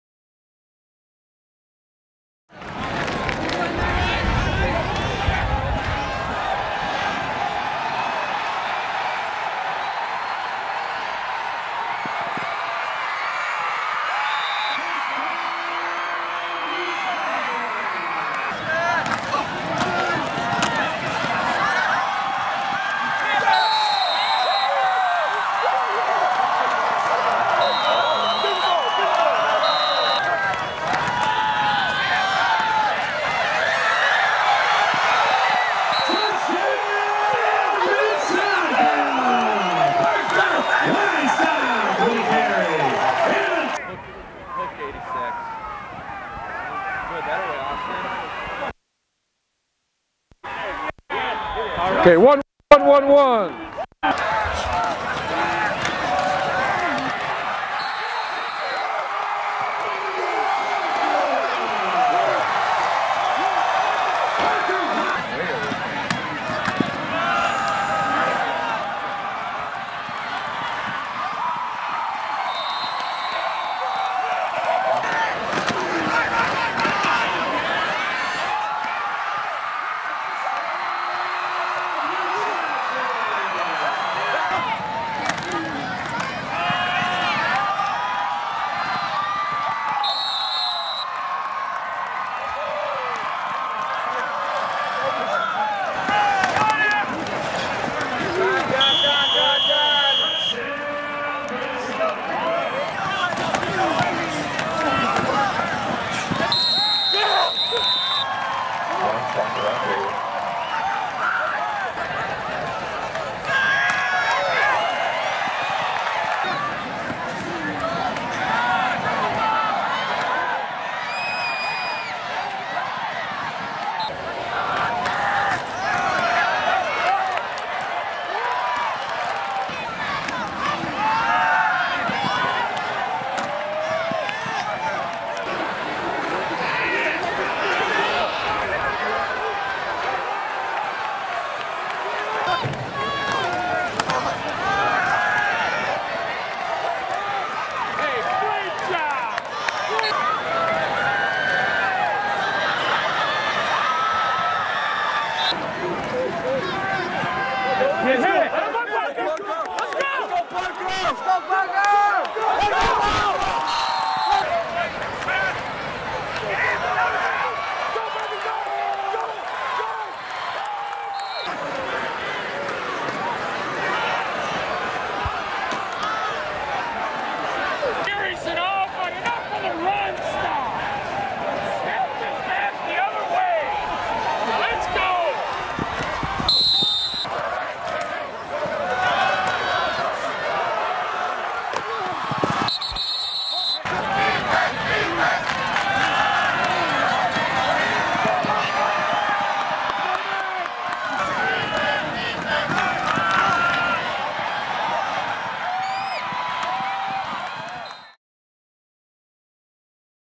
Parker Weinstock Football Highlights 2009
Parker Weinstock football highlights from the 2009 season.